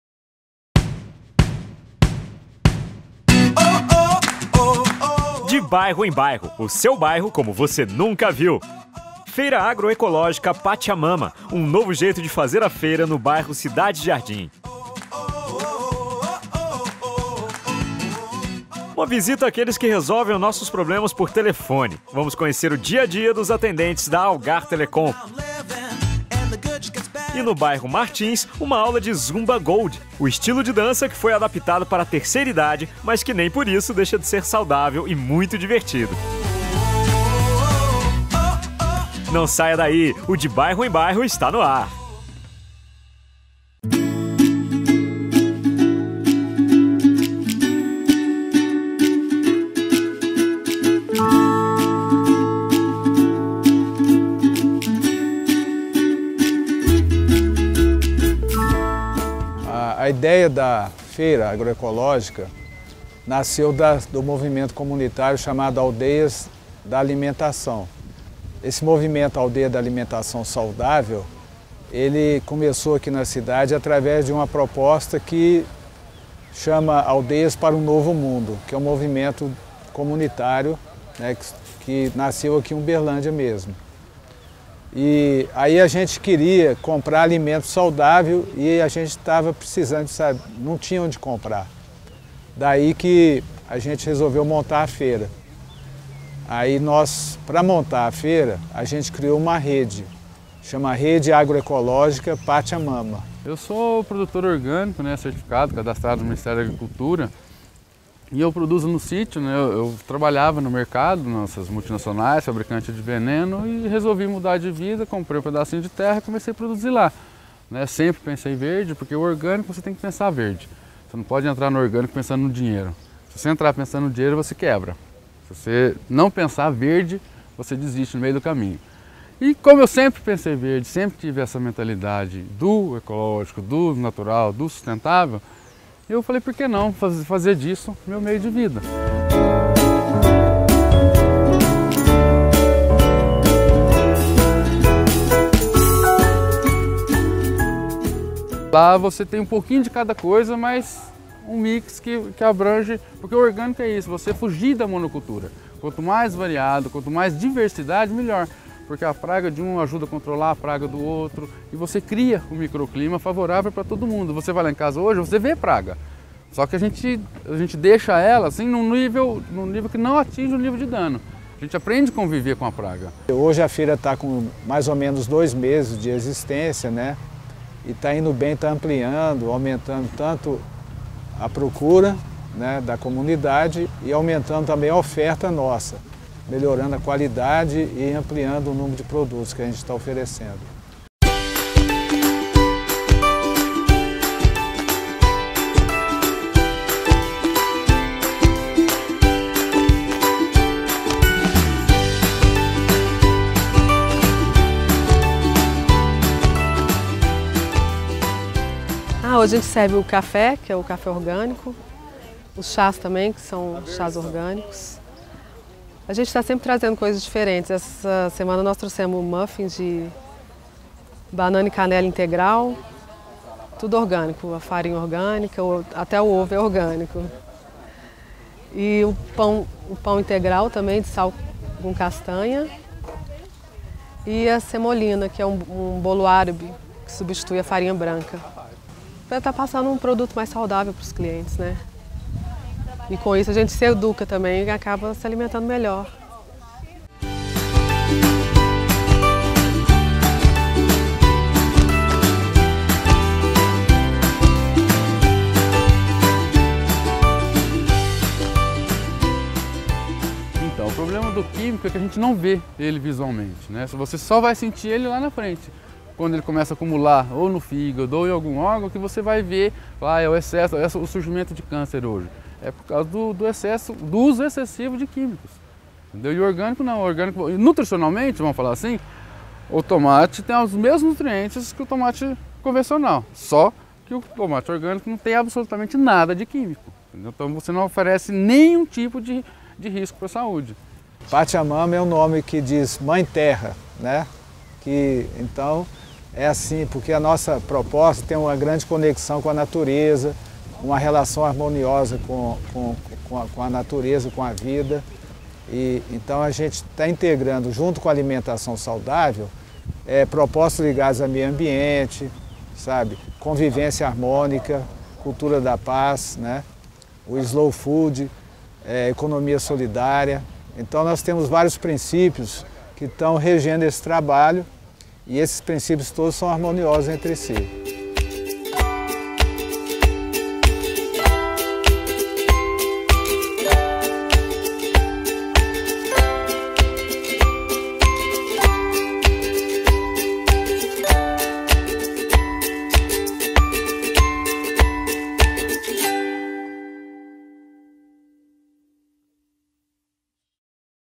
De bairro em bairro [Ep.52] - Feira Agroecológica Patcha Mama
Dia de feira no Bairro Cidade Jardim!  É a feira agroecológica Patcha Mama. Tudo fresquinho e orgânico para o consumidor. É todo domingo a partir das 8h. Vem conhecer um pouco mais desta ideologia de vida.

Entrevistados:

Guilherme Lazzarini - eng. agrônomo
Lourenço Andrade - fundador
Fabiana Rotelli - comerciante